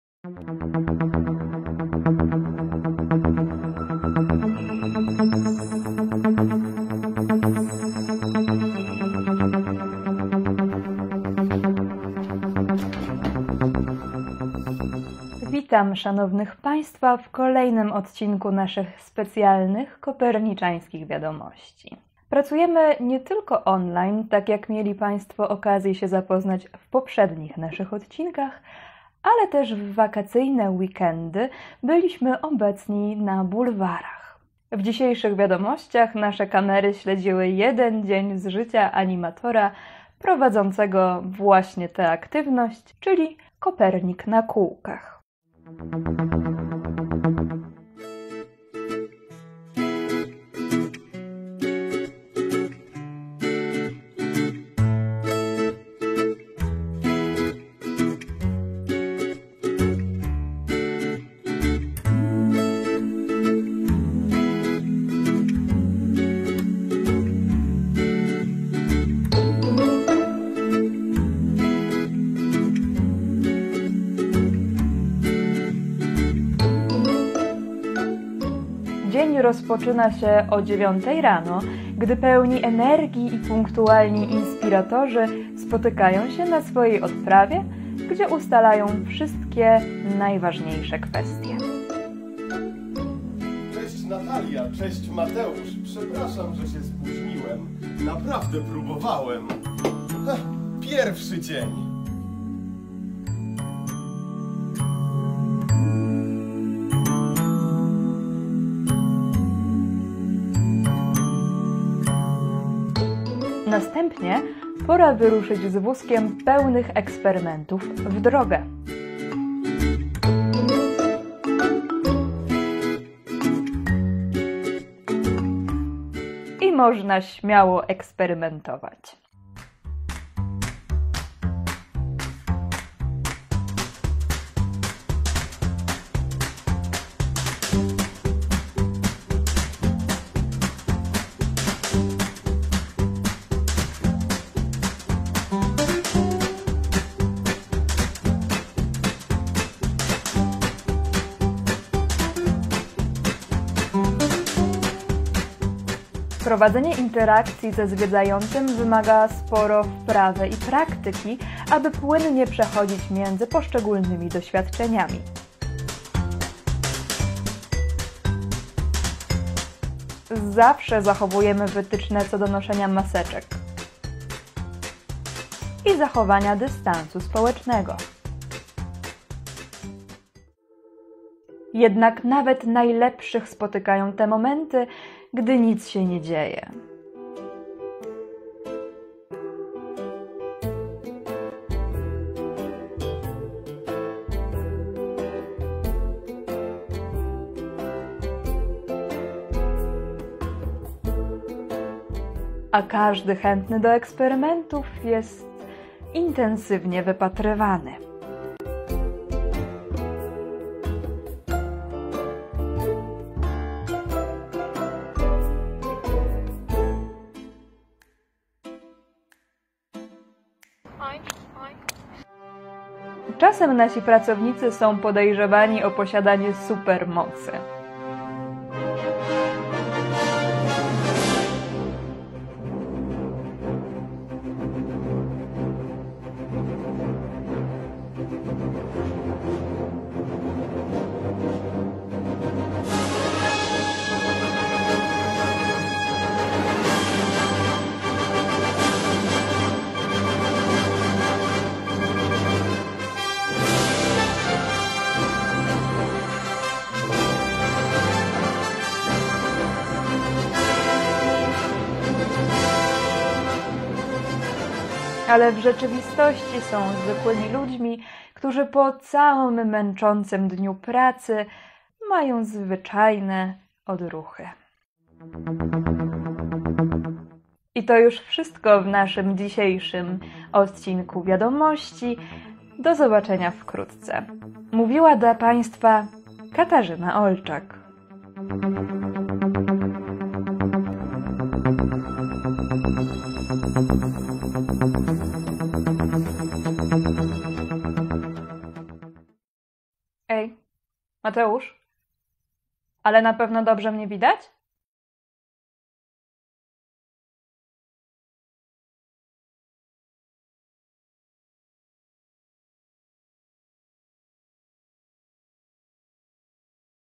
Żarty na kółkach
Uwaga ❗️ News z ostatniej chwili ❗️
Co może zrobić Kopernik na kółkach? 🚗 Odjechać 🙃 ! Obejrzyjcie kolejny odcinek naszych Koperniczańskich wiadomości z przymrużeniem oka i szukajcie nas na bulwarach!